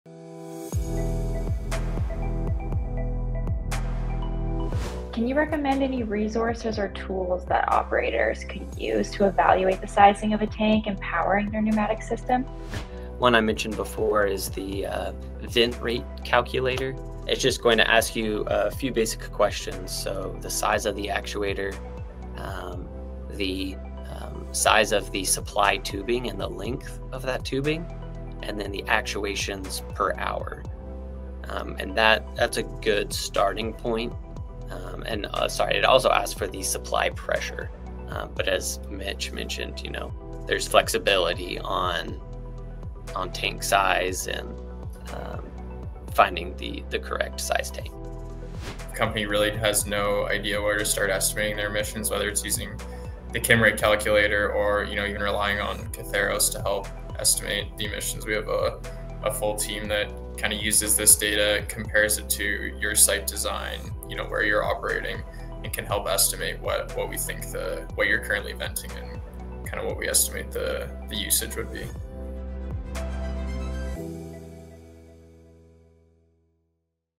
Kimray and Kathairos: Shop Talk Teaser Two
Tune in to the first episode of Shop Talk, Sizing for Zero Emission Pneumatics. Learn from the experts at Kathairos and  @KimrayInc  as we dive into frequently asked questions and topics to that meet the industry’s evolving needs—ensuring efficient, reliable, and hitting emissions goals.

Learn more from Kyle Andrews and Mitch Jones, as our first question dives into where to start if you’re looking at converting your process controls and pumps.